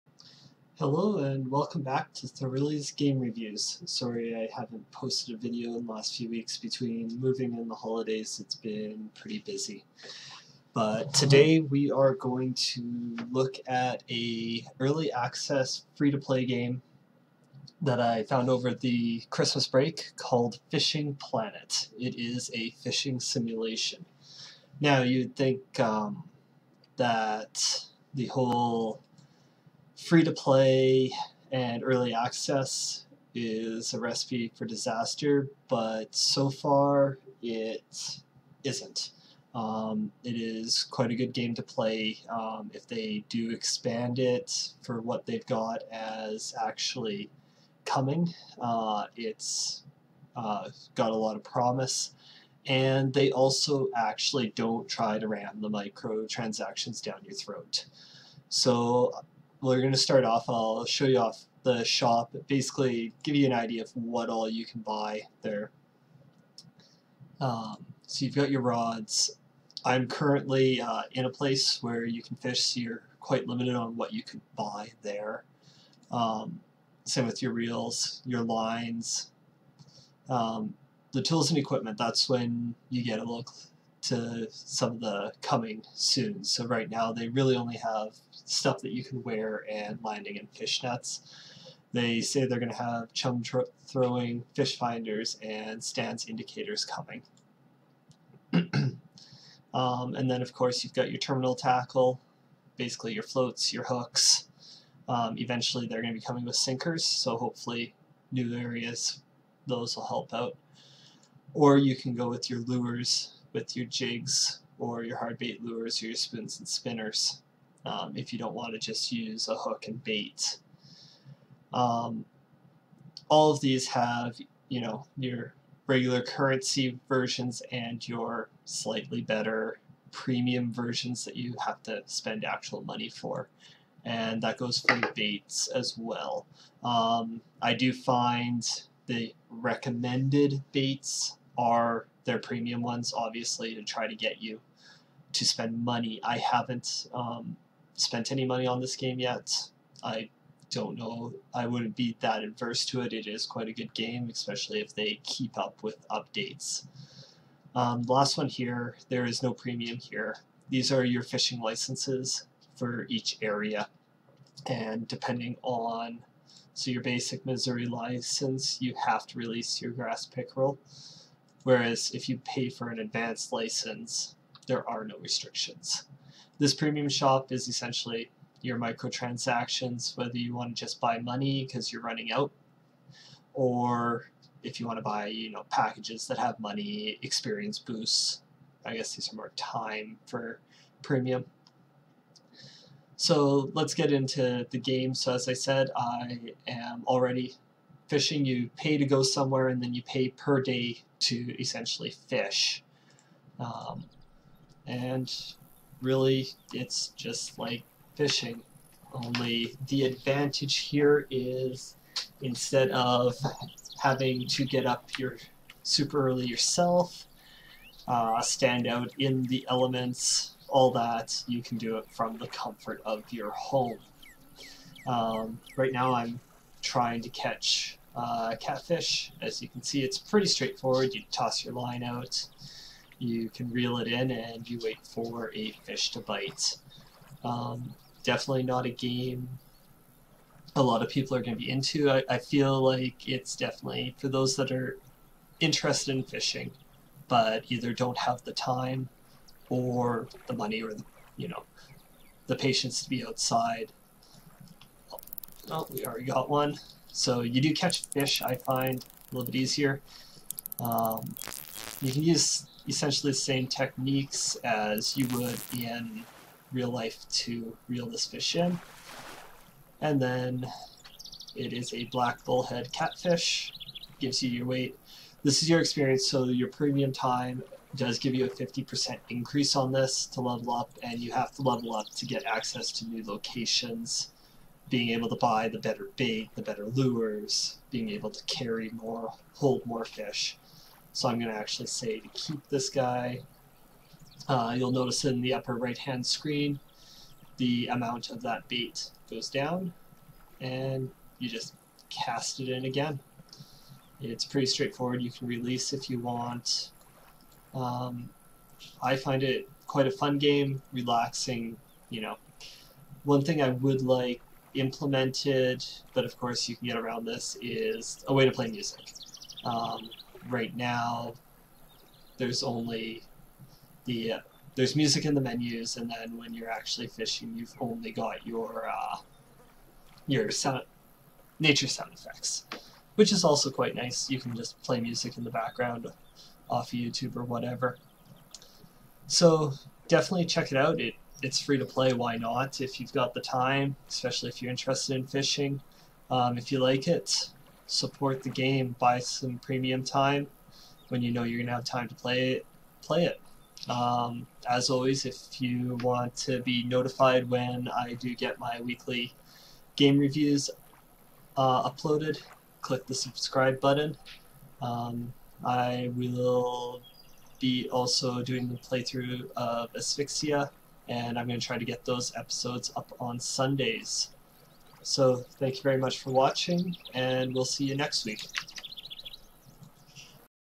Fishing Planet PC Early Access Review
I'm back and this week I review an Early Access Free-to-Play Game.

Fishing Planet.

This is essentially a fishing simulator that is showing a lot of promise.  The possible expansion they have planned is intriguing but the basics make you feel like you are out there fishing.

Game play it is very basic, cast, wait for a bite, reel the fish in.  There are casting rods if you prefer that to sitting watching a float.  The bigger the fish typically the better the experience which helps unlock the better equipment.

Micro-transactions are not rammed down your throat and they are using a model of premium time or buying currency much like World of Tanks.  Also similar to World of Tanks the better bait is premium currency.